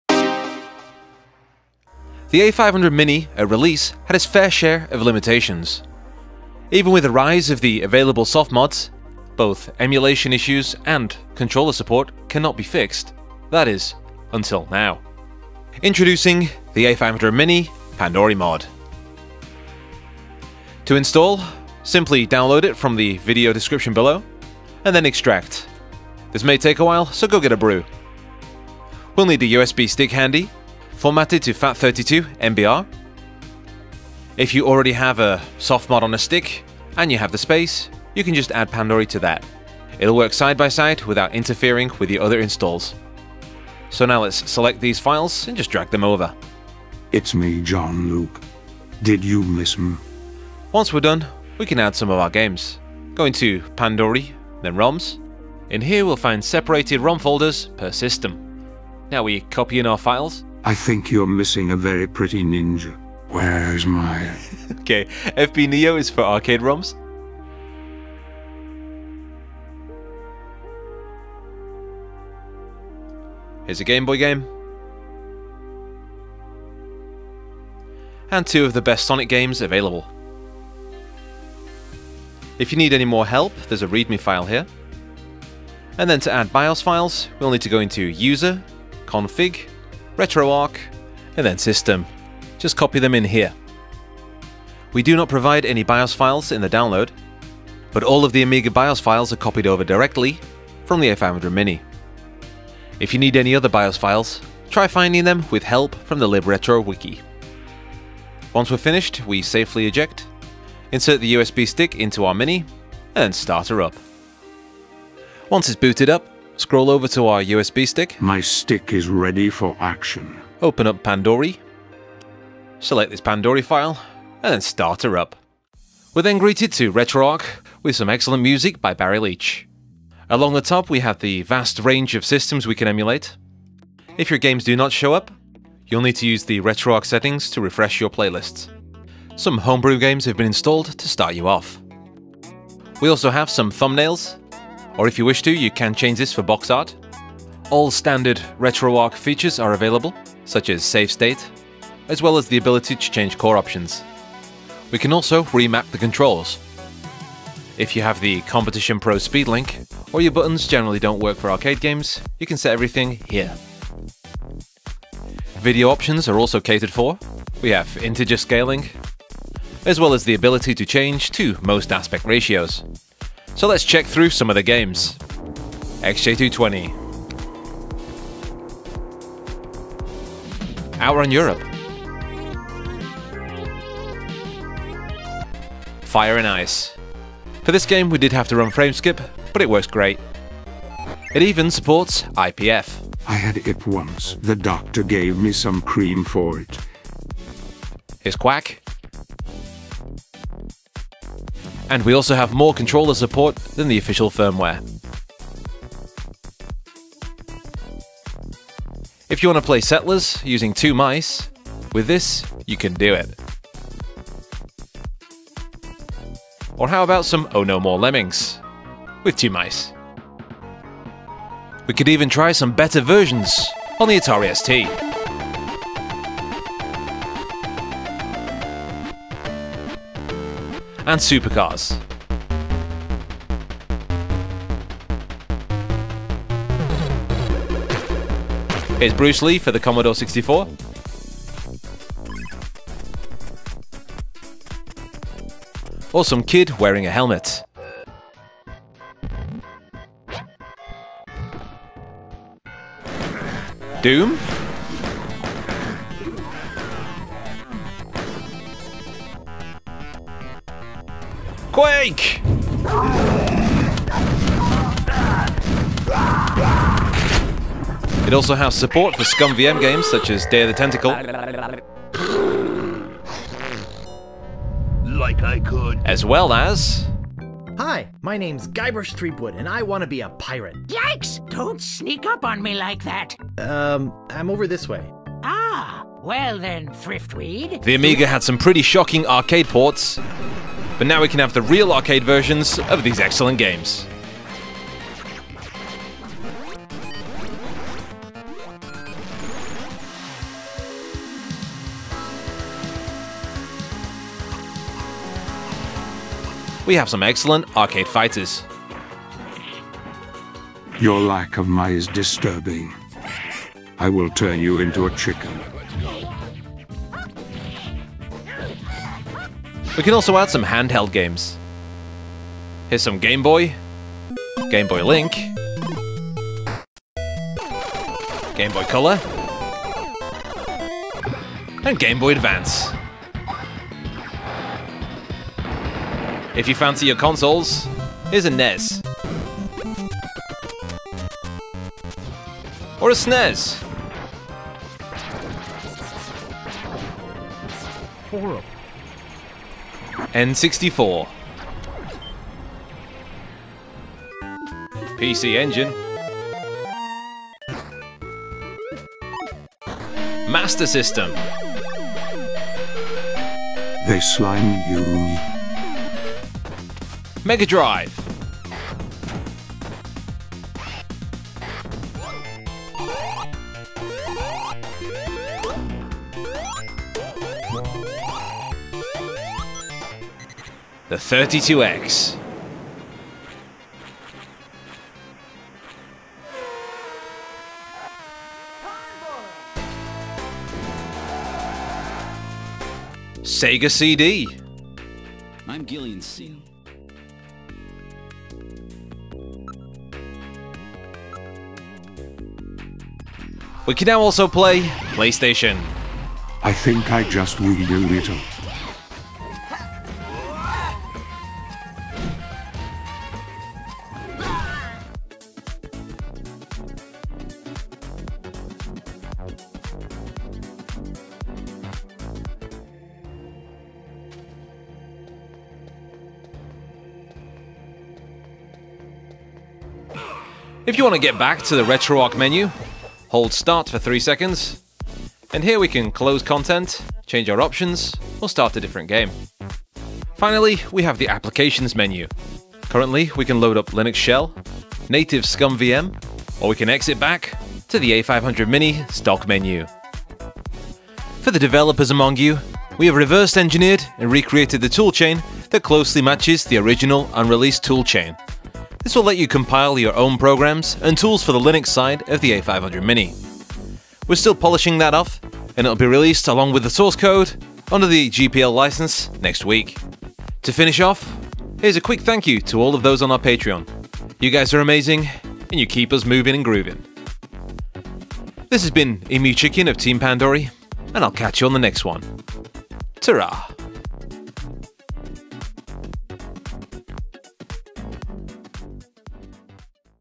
THE A500 Mini Pandory Mod Hack is AWESOME! [Guide & Overview] *RetroArch, New Emulators, Fixes*
Make your Amiga 500 Mini infinitely better with the A500mini Pandory mod!
Download, then pop it on a usb stick.  Easy!

Note: This is a softmod.
It does not change anything on the THEA500MINI.  To remove it, delete the folder or just don't use the USB stick.

Want to buy an A500 Mini?
Sandisk 16gb
8BitDo Brown

Zero Latency Controller
8BitDo Brown Dongle

Timestamps
@0:00- Intro
@0:16- Installing / Setup
@1:55- First Boot / Overview
@2:56- Game Testing / Amiga
@3:44- Game Testing / Other Computers
@4:48- Game Testing / Arcade
@5:20- Game Testing / Handheld
@5:38- Game Testing / Console
@7:15- Applications Menu
@7:30- Closing / Thanks